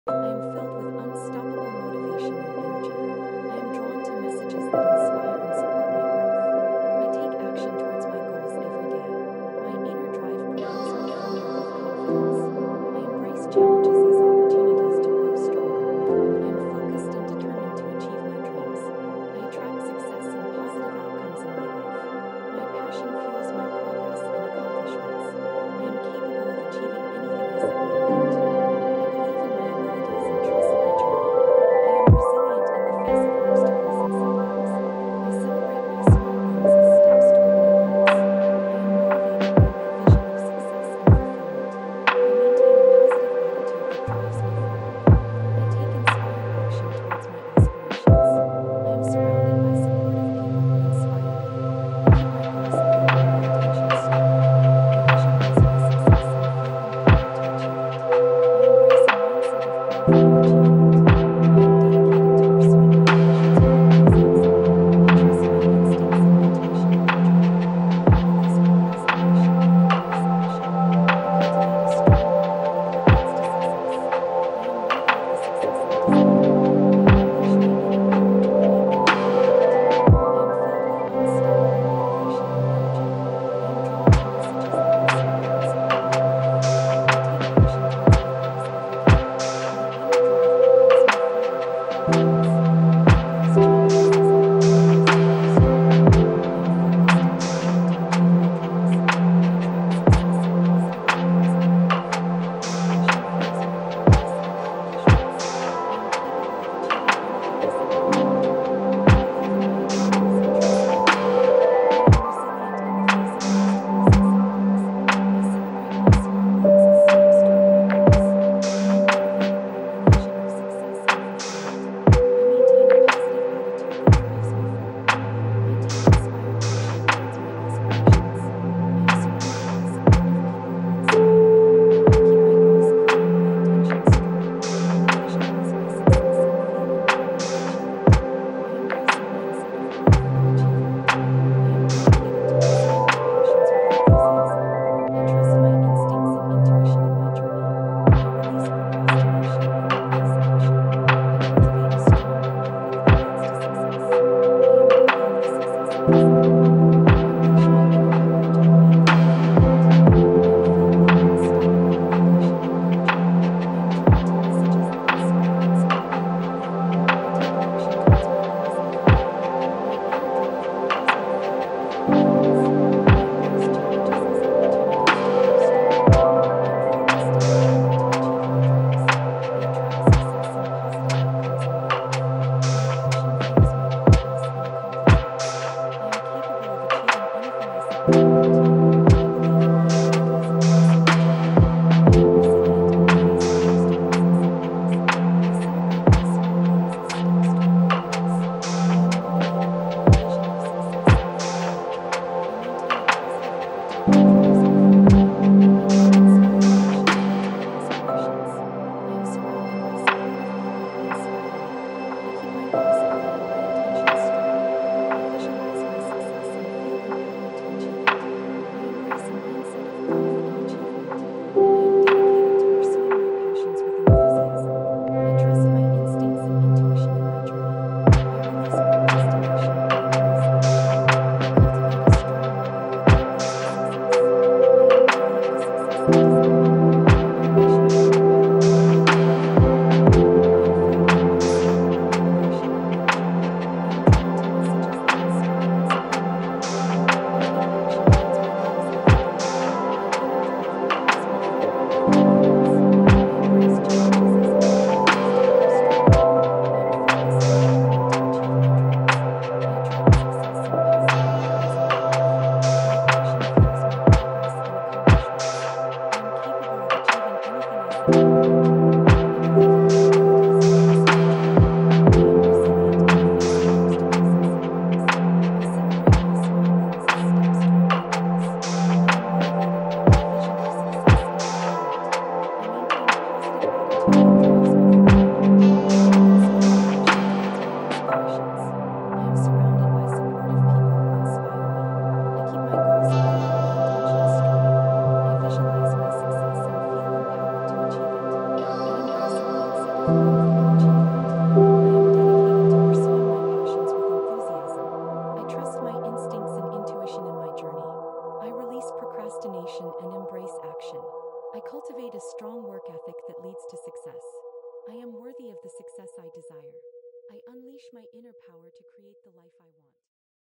supercharge motivation | unlock your inner power
This video found you for a reason—you’re ready for the change you’ve been seeking. Deep down, you know you're capable of more. Let this subliminal unlock the doors to your desires.

Through gentle, subconscious reprogramming, you’ll begin to:

☆ Attract the positive energy and experiences you're ready for.
☆ Shift limiting beliefs into empowering thoughts.
☆ Align your mind with the life you truly want to live.
☆ Listen with or without headphones, let go of any doubts, and trust that you are exactly where you need to be. Your transformation has already begun.

This video found you for a reason—now it’s time to discover why.

Affirmations used:

♡ I am filled with unstoppable motivation and energy.
♡ I am drawn to messages that inspire and support my growth.
♡ I take action towards my goals every day.
♡ My inner drive propels me forward with confidence.
♡ I embrace challenges as opportunities to grow stronger.
♡ I am focused and determined to achieve my dreams.
♡ I attract success and positive outcomes in my life.
♡ My passion fuels my progress and accomplishments.
♡ I am capable of achieving anything I set my mind to.
♡ I believe in my abilities and trust my journey.
♡ I am resilient in the face of obstacles and setbacks.
♡ I celebrate my small wins as steps toward my goals.
♡ I am motivated by my vision of success and fulfillment.
♡ I maintain a positive attitude that drives me forward.
♡ I take inspired action towards my aspirations.
♡ I am surrounded by supportive people who inspire me.
♡ I keep my goals clear and my intentions strong.
♡ I visualize my success and feel empowered to achieve it.
♡ I embrace a mindset of growth and achievement.
♡ I am dedicated to pursuing my passions with enthusiasm.
♡ I trust my instincts and intuition in my journey.
♡ I release procrastination and embrace action.
♡ I cultivate a strong work ethic that leads to success.
♡ I am worthy of the success I desire.
♡ I unleash my inner power to create the life I want.

Remember, you're capable of amazing things, and the universe is here to cheer you on every step of the way!

❀ Thanks for being part of the BlendEd Insights family! ❀